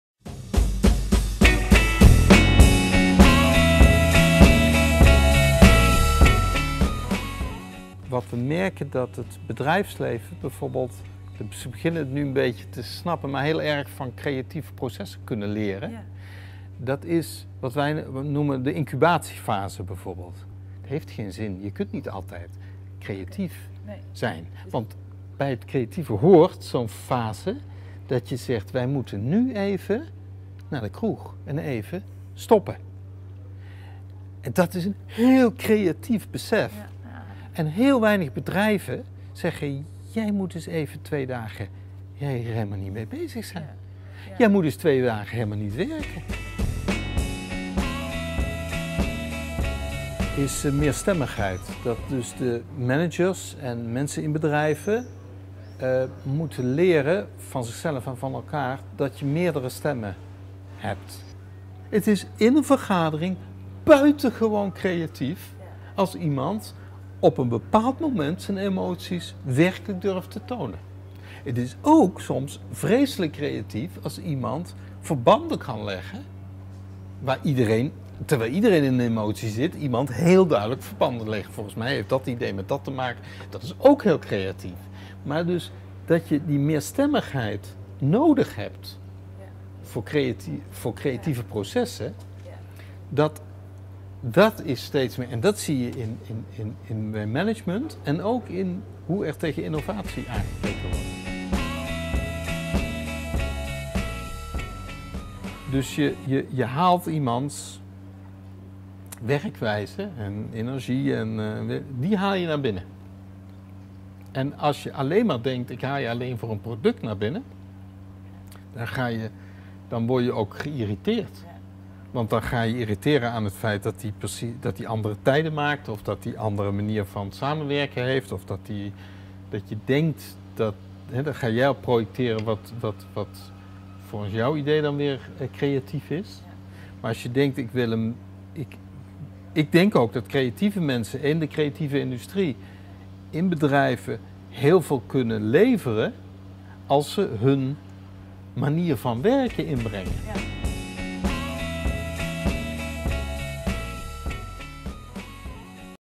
Wat kunnen organisaties leren van het creatieve proces?
Deze video geeft antwoord op de vragen:
- Wat kunnen managers leren van het creatieve proces?
- Wat kunnen organisaties leren van de creatieveling?

interview met Nirav Christophe, lector aan de Hogeschool van de Kunsten, Utrecht